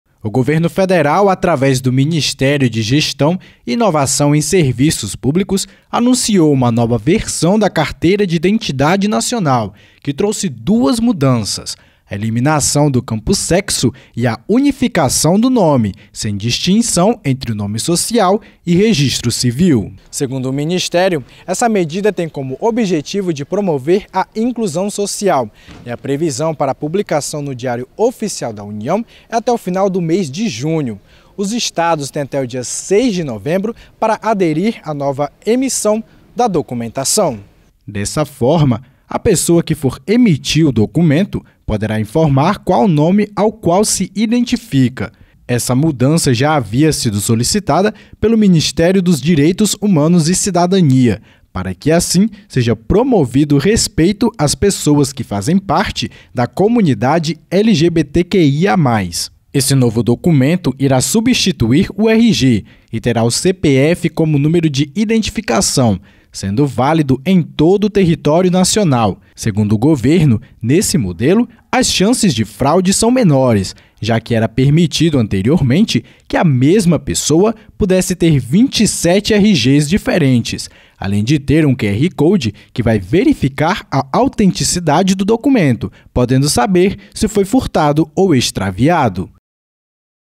Carteira de identidade passa por mudança e não terá campo "sexo": confira alterações | SBT Altamira
Foi publicado pelo Governo Federal na última sexta-feira (19), que os campos "sexo" e "nome social" serão extintas da nova Carteira de Identidade Nacional